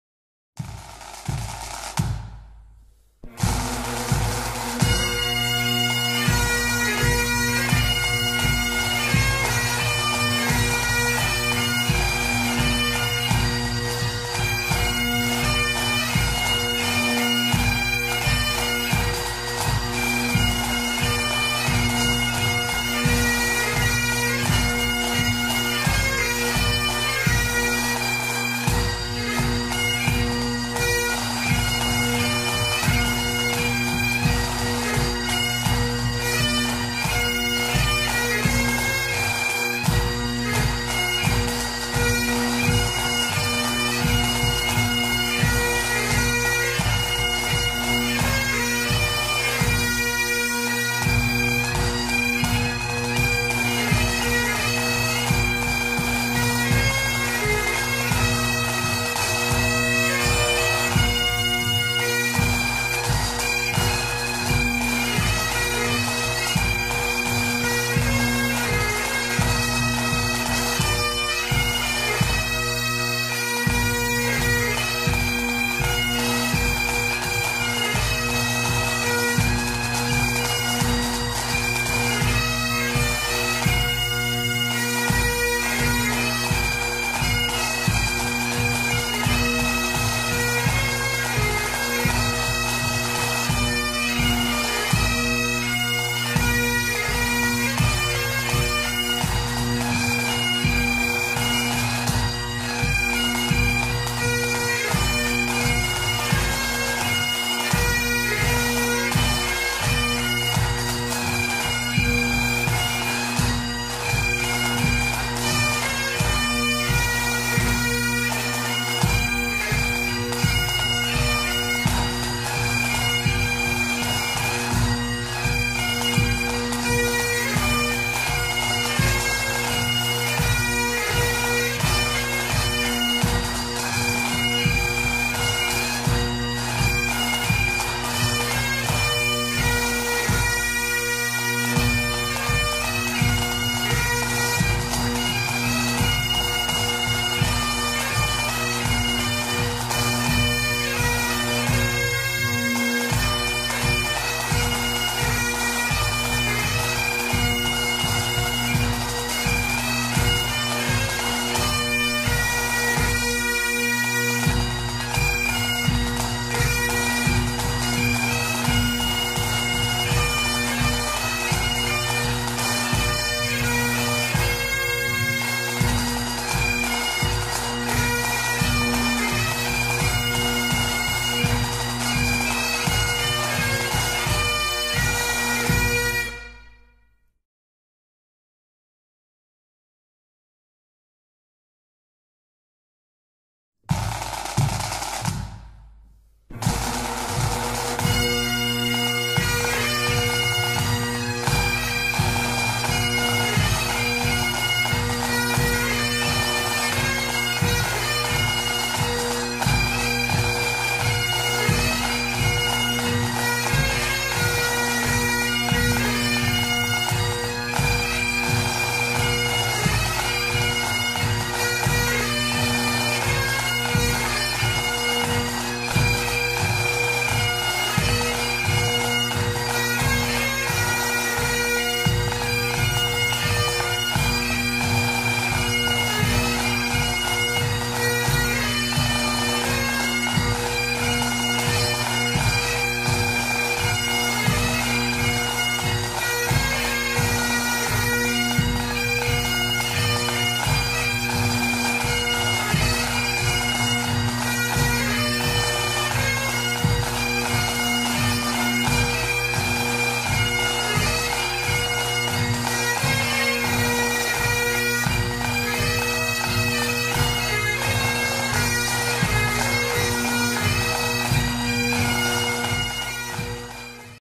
The CLAN GUNN
Clan Gunn is the world's oldest clan of Scotland dating back the days of the Viking age.